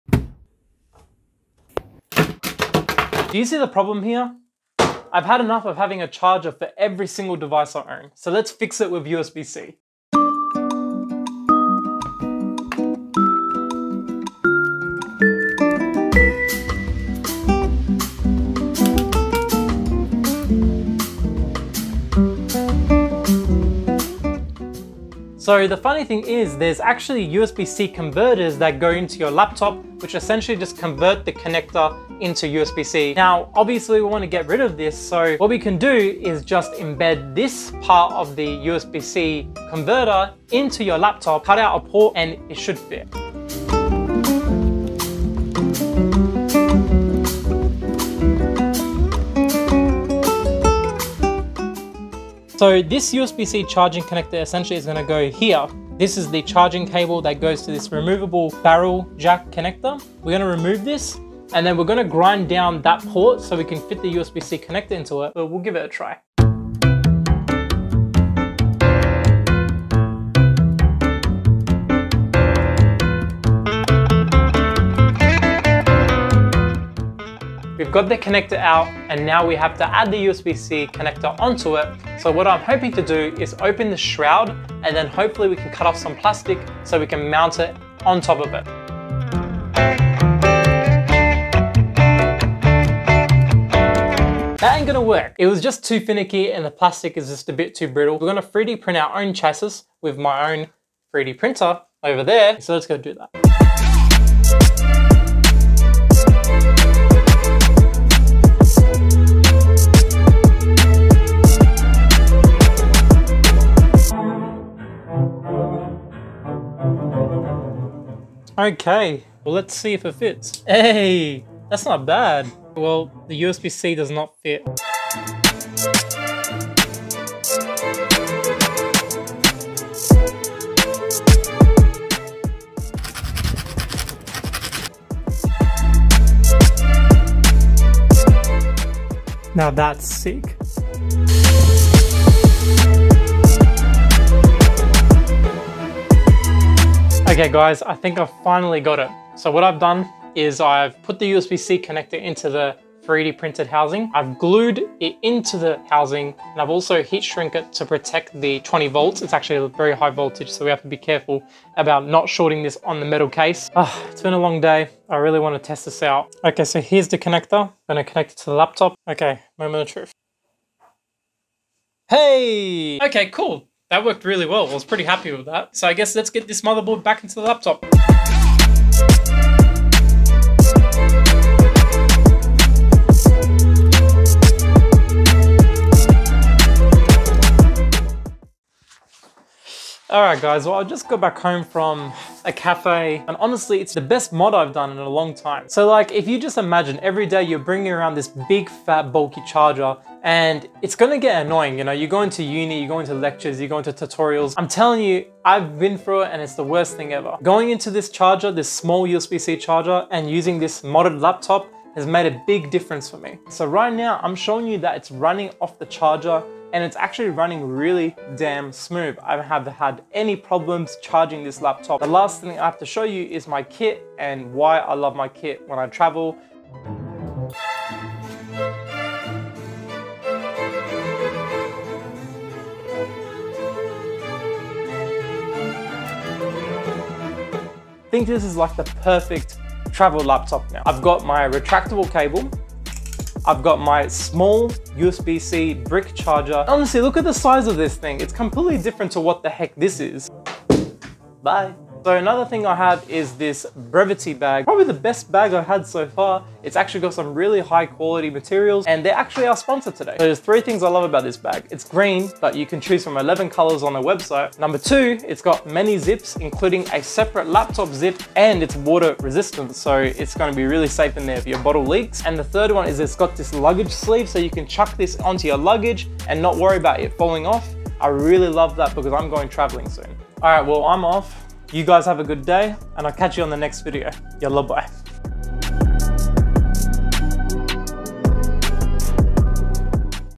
Hacking my laptop so I could travel lighter!
You can mod/hack your laptop with USB-C so you can ditch the bulky laptop charger and use these smaller USB-C PD laptop chargers. Let's change that!

Laptop Gear:
USB-C Laptop Charger
Belkin 68W Laptop Charger
Retractable USB-C Cable
100W Compatible USB-C Cable
Laptops with USB-C Charging (USB-PD)

Laptop Mod:
Laptop Barrel Jack to USB-C Converter

CAUTION:
1. You must find the right barrel jack to USB-C converter for your laptop that has the same voltage marked. Do not buy any random one to avoid potentially damaging your laptop due to different laptop voltages. Most laptops support between 18V-20V. Look on the back of your laptop and confirm the voltage with the adapter you are purchasing. The USB-C adapter internals my look different to mine so please bear that in mind.
2. You must use a USB-C Charger that is equal to or more than the wattage of your original charger (which is typically more than 65W). You cannot use a typical phone charger to charge your laptop because it does not have enough power. You must use a USB-C to C cable that supports charging, and not USB-A to C.
3. Be sure to properly protect any bare wires or pads with heatshrink, glue or tape. You do not want any electrical hazards here nor airport customs to annoy you for your dodgy laptop. Do it right the first time. Do not use hot glue, it will melt from the heat while charging.

0:00 - The Problem - Bulky Chargers.
0:25 - The Solution - USB-C Adapter (Easy Way and Hard Way)
0:52 - The Plan - Hard Way
3:08 - The Result

Thank you to (prod.Codec) for making the amazing remixed classical tracks (Bach and others) -